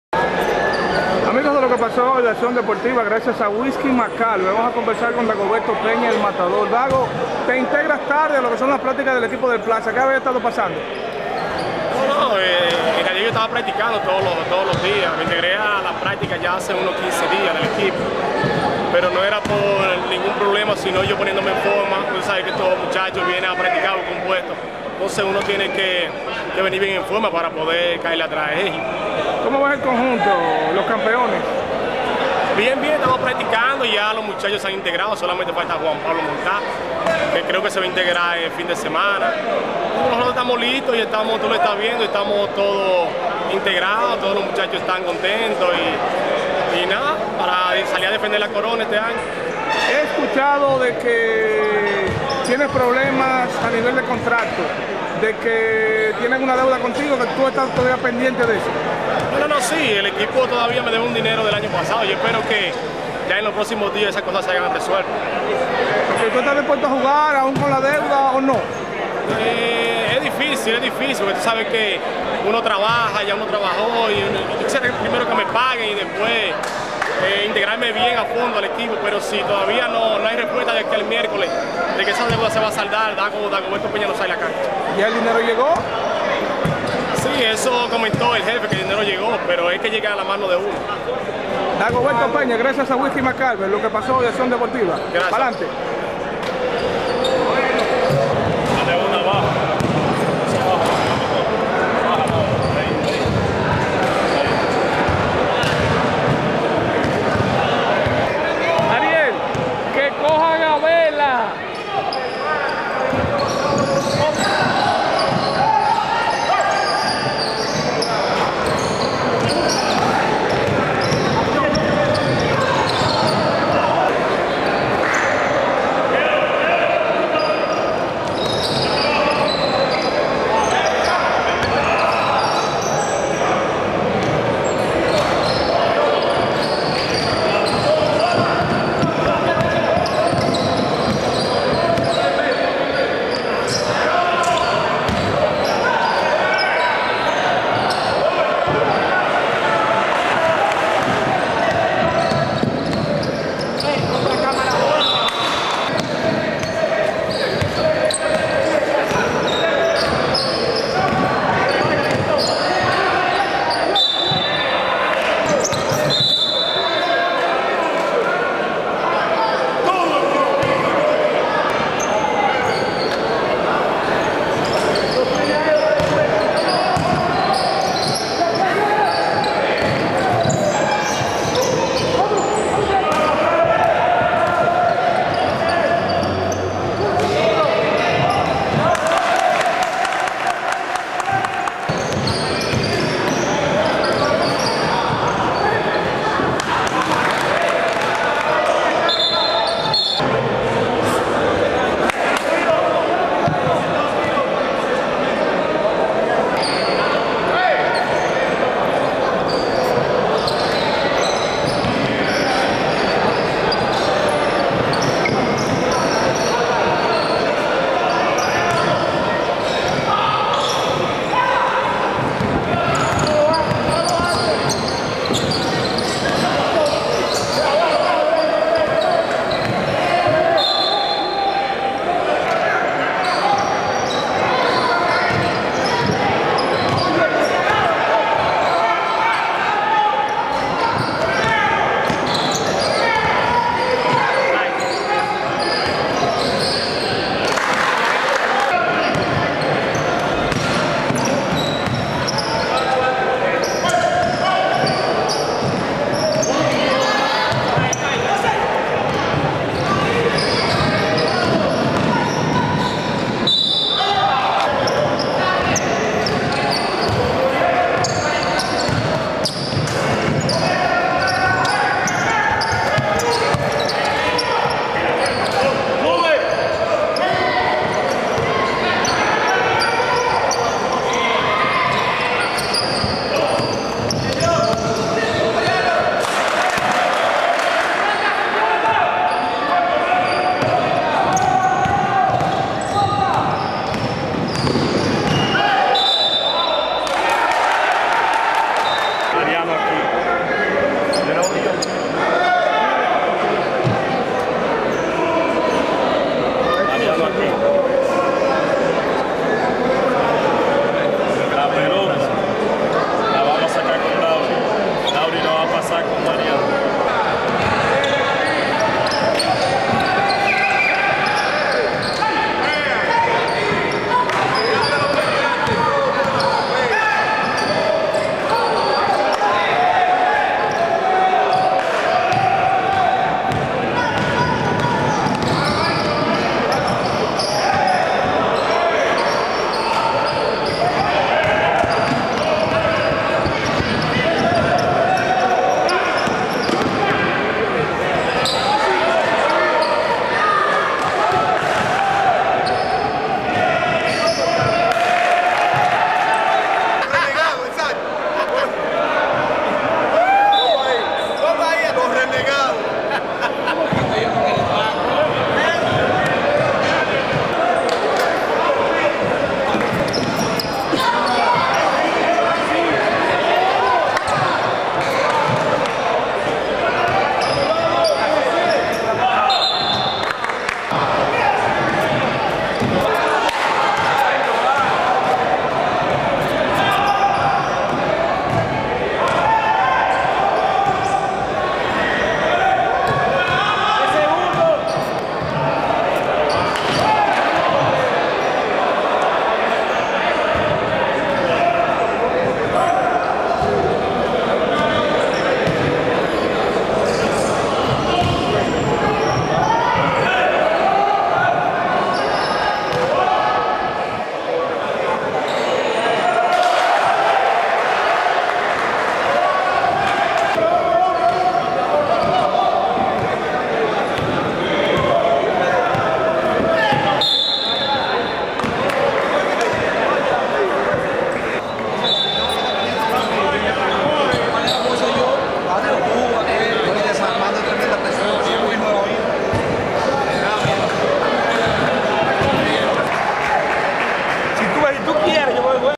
DAGOBERTO PEÑA HABLA DE DEUDAS Y PAGOS, PLAZA VENCIO CDP FOGUEO.wmv
PLAZA UTILIZO A EDWARD SANTANA, JOSE OLIVERO, LUIS CABRERA Y DAGOBERTO PEÑA.  EL HOMBRE GRANDE LO FUE JUAN CARLOS SANTANA EN LA VICTORIA ANTE  CDP EN JUEGO BENEFICO A FAVOR DE ARMANDO BETEMIT, QUIEN A SU VEZ REALIZO EL SAQUE DE HONOR.